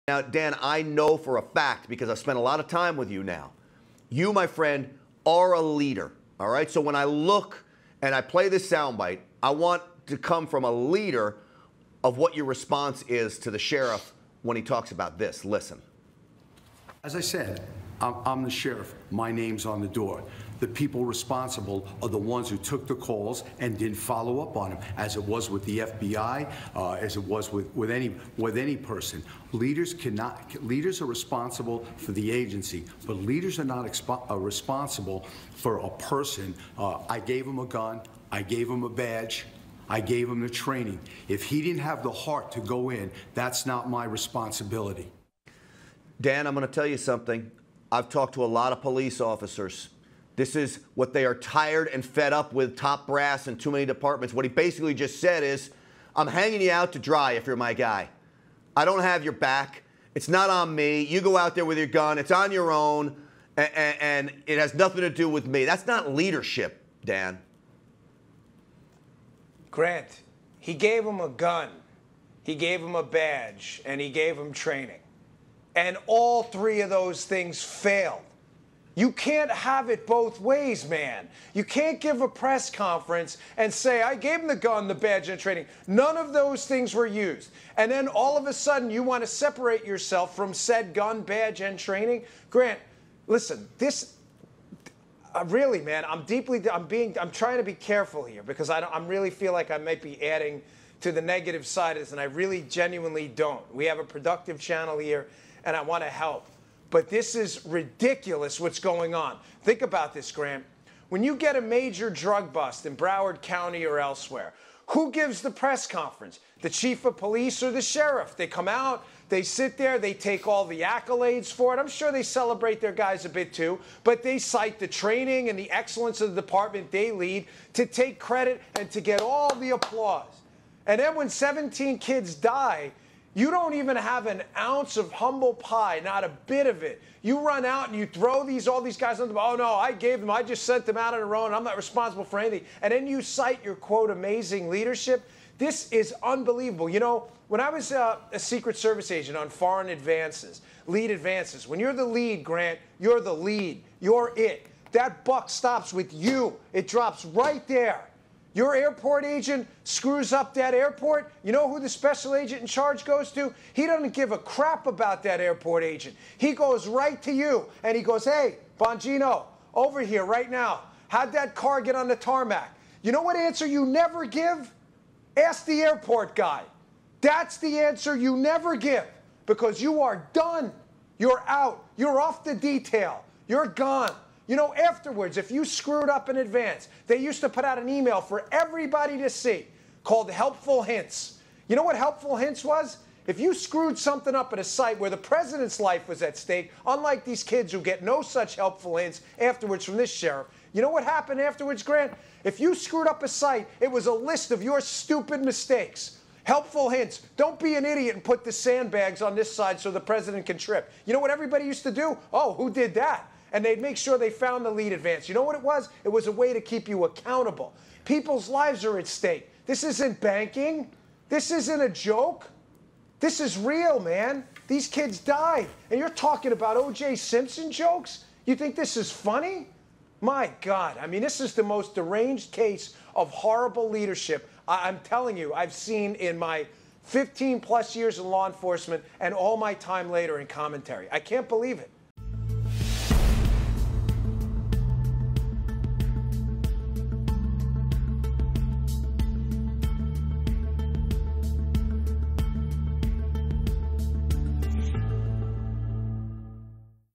Dan Bongino: Sheriff Refuses to Take Responsibility
"People's lives are at stake. This isn't banking, this isn't a joke. This is real man. These kids died and you are talking about O.J. Simpson jokes. You think this is funny? This is the most deranged case of horrible Ieadership...I've seen in my 15 plus years of law enforcement and all my time later in commentary." —Dan Bongino